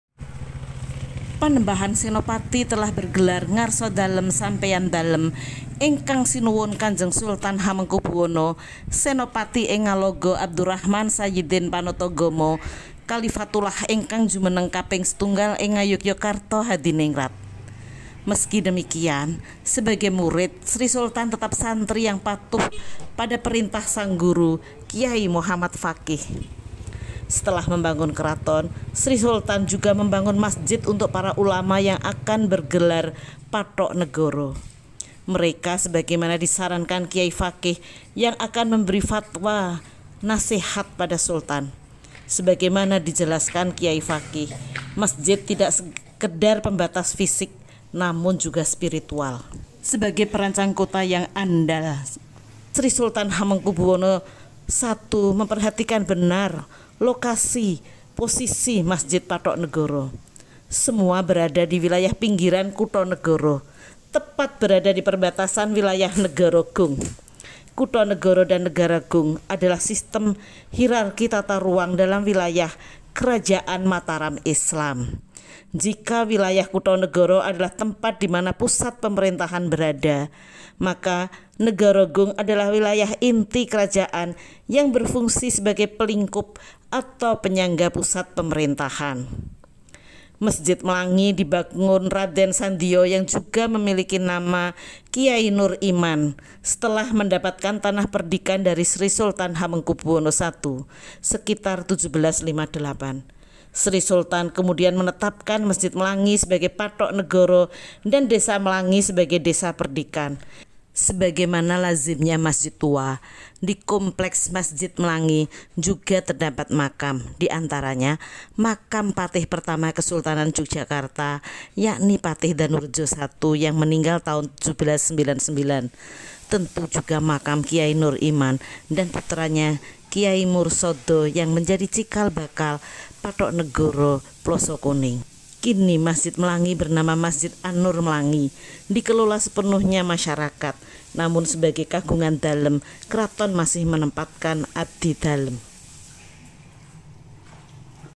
Masjid Pathoknegara MLANGI
Masjid Pathoknegara Mlangi dibangun Kiai Nur Iman atau Raden Sandiyo yg tidak lain adalah kakak Pangeran Mangkubumi. setelah Mangkubumi dinobatkan sebagai Sultan Ngayogyakarta dan bertahta di Kraton Yogyakarta, Kiai Nur Iman diberi tanah perdikan lalu membangun. masjid.
Memenuhi nasehat gurunya, Kiai Faqih Sultan HB I mengangkat Kiai Nur Iman sebagai Pathoknegara dan masjidnya menjadi Masjid Pathoknegara Mlangi.
Kini masjid sudah mengalamai perubahan total. Meski demikian, statusnya tetap masjid Kagungan Dalem dan ada Abdi Dalem Pathoknegara yg ditempatkan. Jogja istimewa kan. masjid ini bisa menjadi destinasi wisata reliji atau wisata masjid juga. istimewahnya jogja.